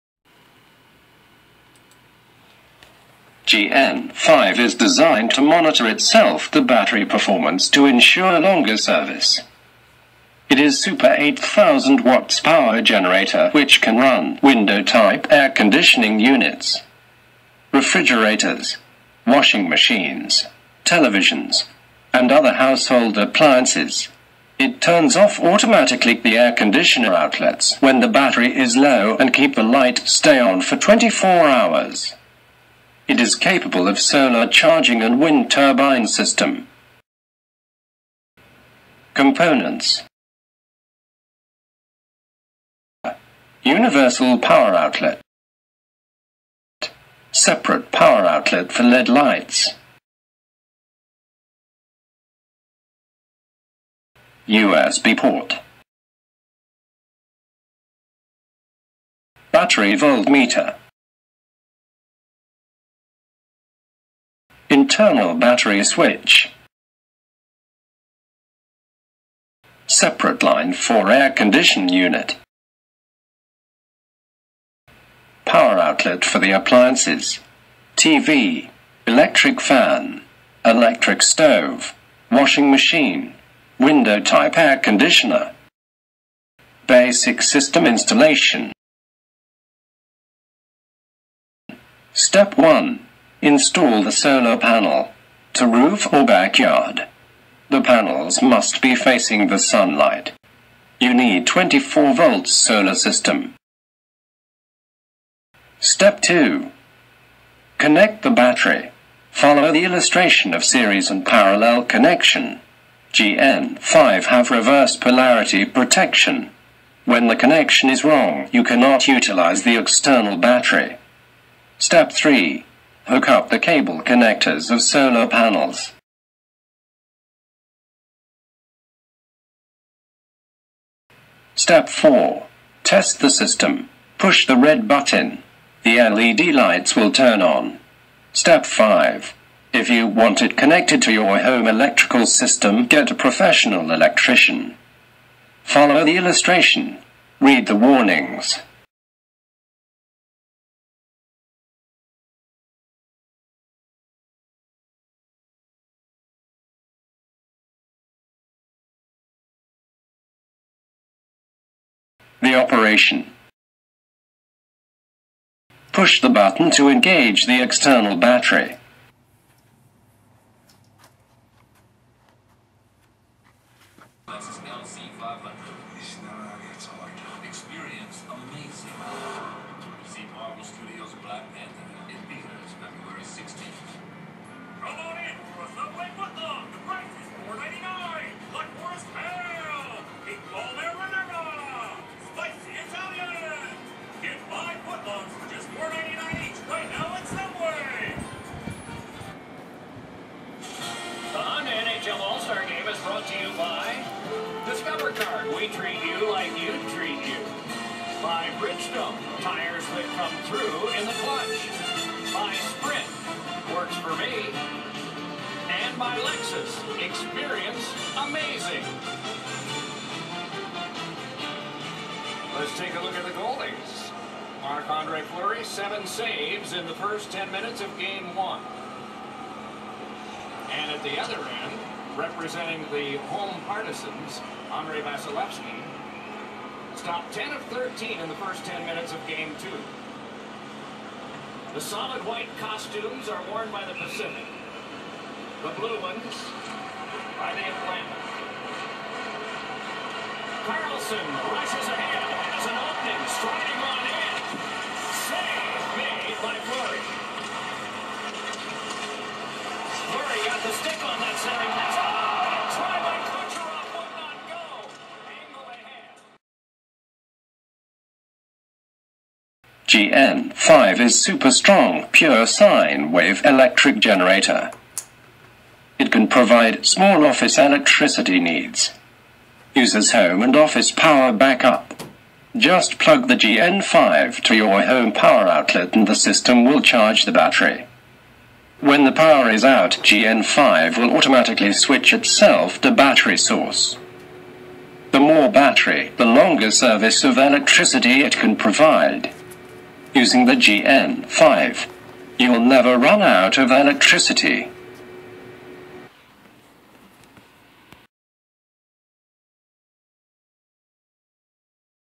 8000 Watts Solar Power Generator GN5
GN5 is fully automatic 8kw power Solar Generator that keeps you house 24/7 free energy. The system turns off Air condition outlet automatically if the battery level is 50% and turns off refrigerator AC outlets automatically at when the battery level is 75% to keep light power supply running 24 hours. All system outlet reset automatically during daytime when sunlight is charging the battery.